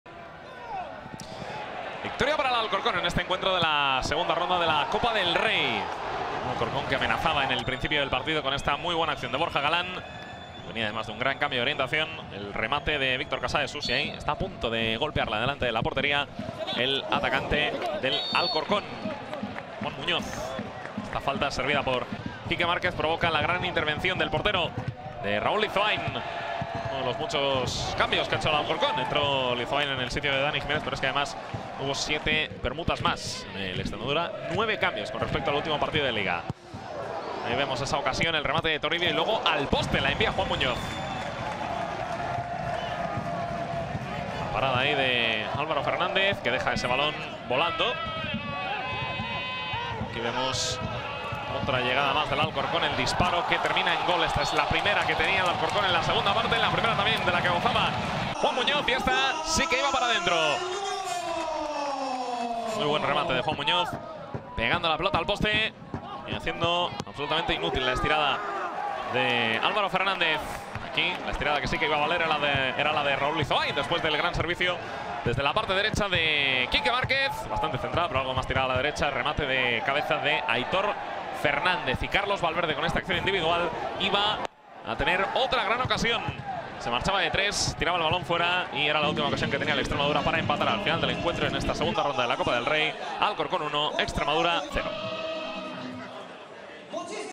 Resumen de AD Alcorcón vs Extremadura UD (1-0)
La AD Alcorcón ganó al Extremadura UD con gol de Juan Muñoz y pasa a la siguiente eliminatoria de La Copa del Rey 2018/2019